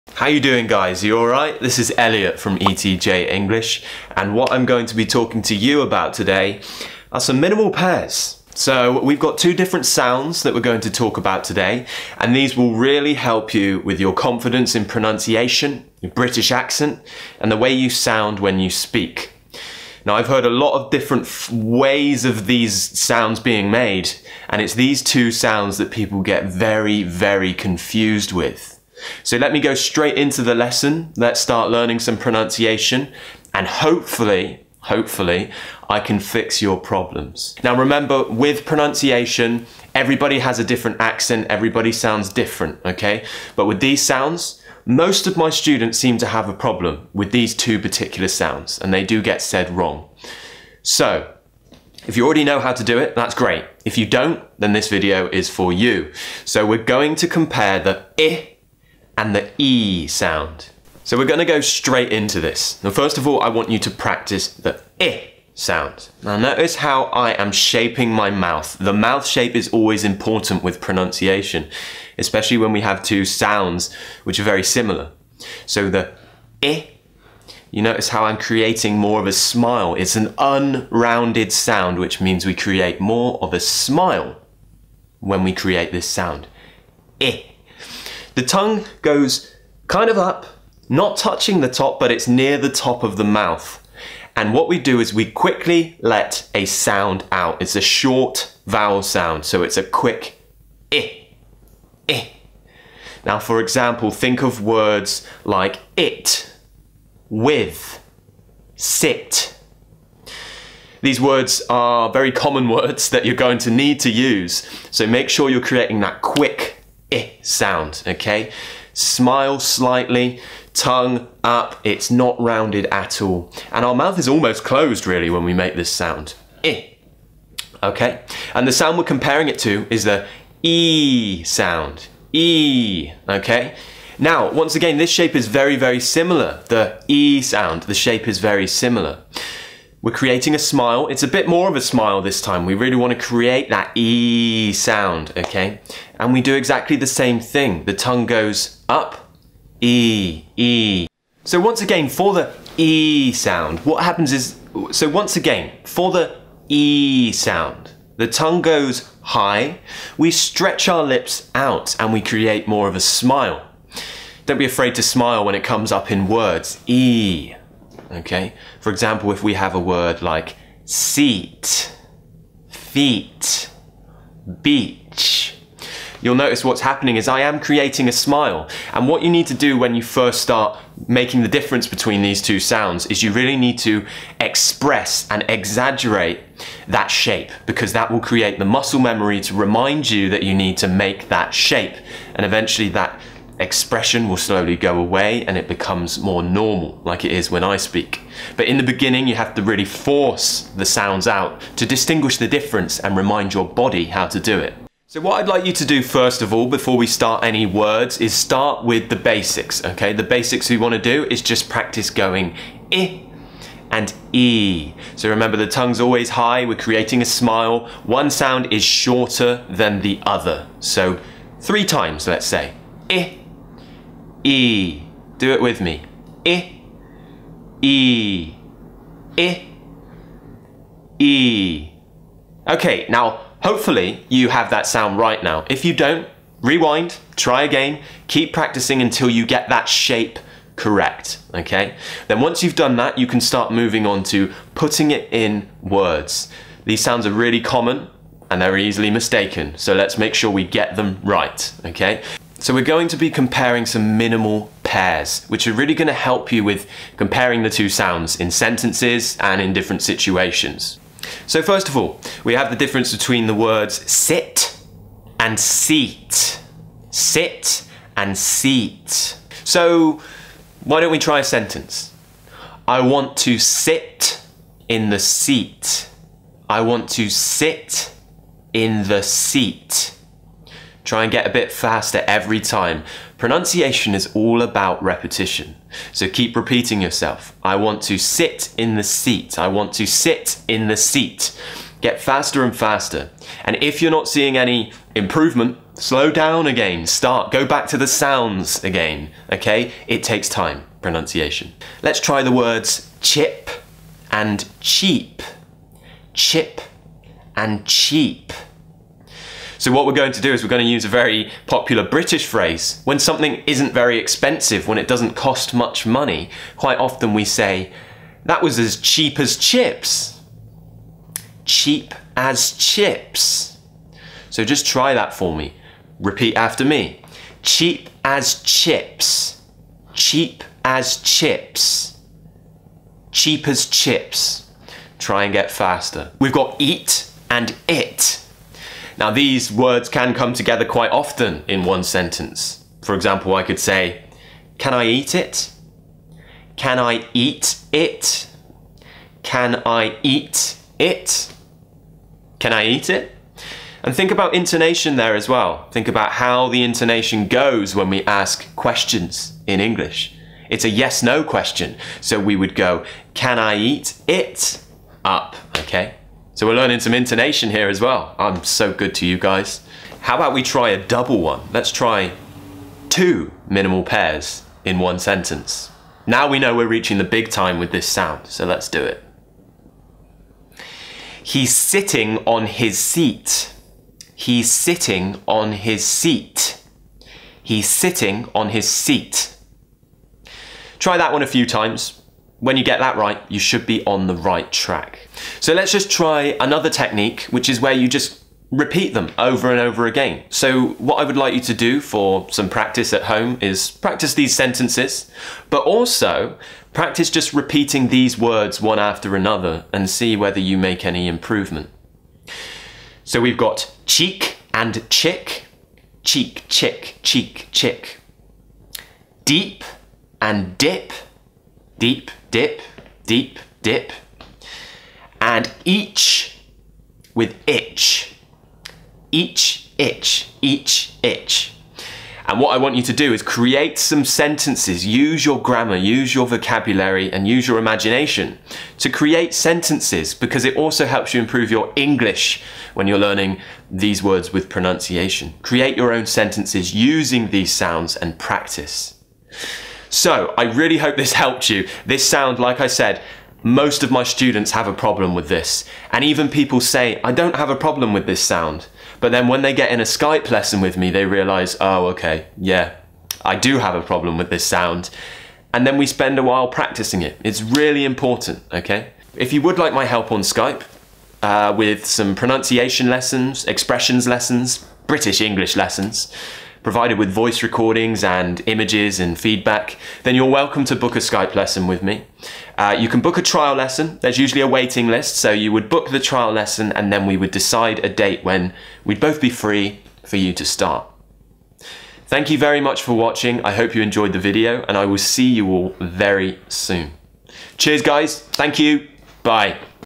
English Pronunciation: /i/ Ship vs. /i:/ Sheep
Remember, you don't NEED an RP British accent to speak English well... There are loads of accents out there! I'm just here to help people who have a love for modern versions of RP.

British English pronunciation lesson all about these two similar sounds! /i/ and /i:/ - one is a short vowel, and the other is a long vowel.  All of my Skype students have a problem with distinguishing the difference between these sounds, so I thought I'd try to help you all out for free in this British English pronunciation lesson.